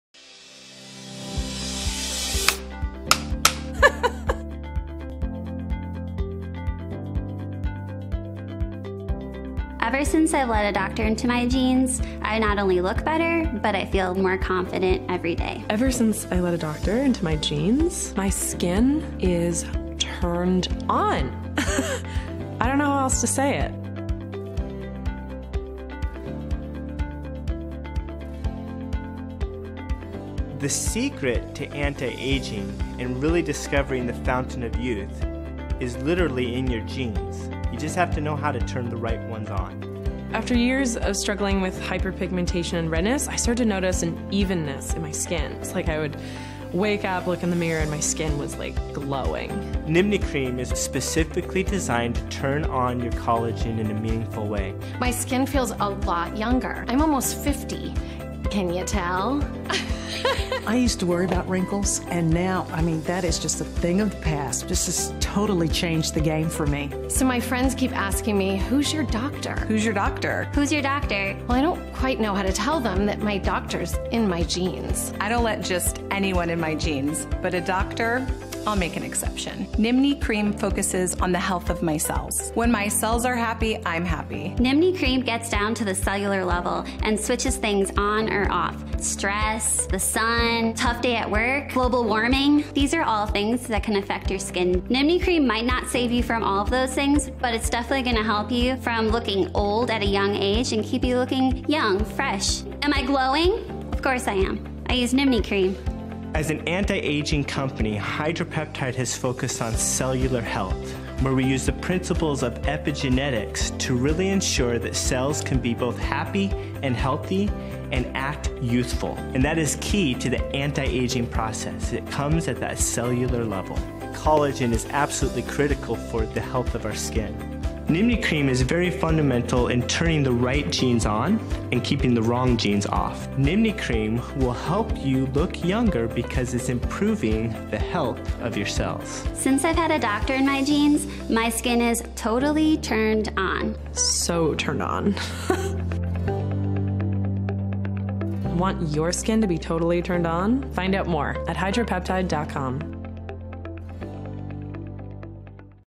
HydroPeptide Nimni Cream
- Je založen na revoluční studii kolagenu Dr.Marcela Nimni a obsahuje pomalu se uvolňující retinol, který zajišťuje omlazení kůže a redukci drobných linek a vrásek.
- Pomáhá zvýšit plnost a elasticitu
- Díky exkluzivnímu patentovanému aminokyselinovému komplexu pomáhá optimalizovat produkci kolagenu.
- Je vhodný pro necitlivou pleť s mimickými vráskami a ztrátou objemu.